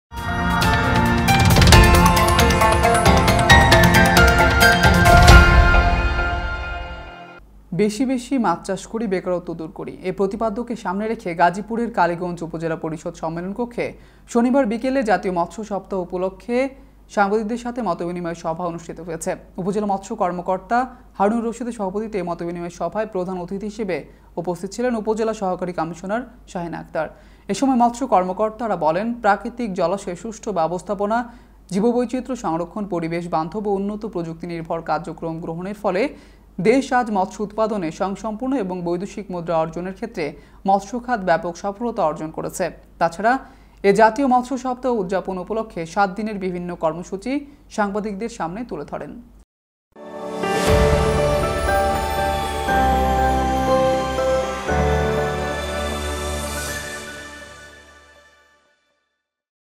কালীগঞ্জে জাতীয় মৎস্য সপ্তাহ উপলক্ষে সাংবাদিকদের সাথে মতবিনিময় সভা
🌟 বিস্তারিত: বেশি বেশি মাছ চাষ করি বেকারত্ব দূর করি,এই প্রতিপাদ্যকে সামনে রেখে গাজীপুরের কালীগঞ্জ উপজেলা পরিষদ সন্মেলন কক্ষে  শনিবার বিকেলে জাতীয় মৎস্য সপ্তাহ ২০২১উপলক্ষে সাংবাদিকদের সাথে এক মতবিনিময় সভা অনুষ্টিত হয়েছে।

🌟  যোগাযোগের ঠিকানা:
Bangla Channel